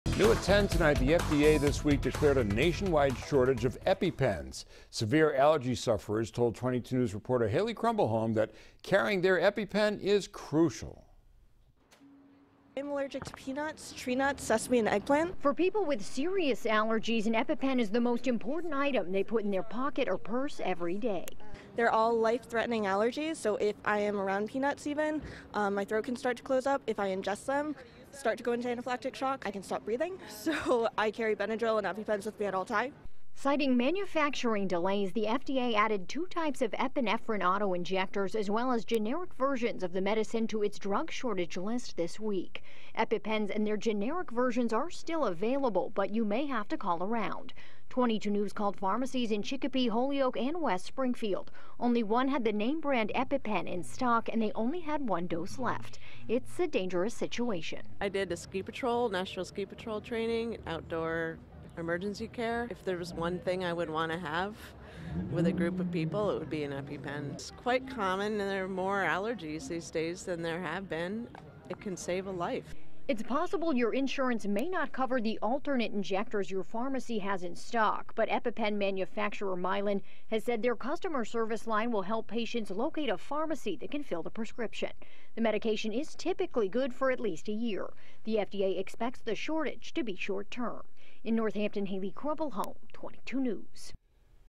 Nation-wide shortage of EpiPens a concern for people with deadly allergies
For people with serious allergies, an EpiPen is the most important item they put in their pocket or purse every day.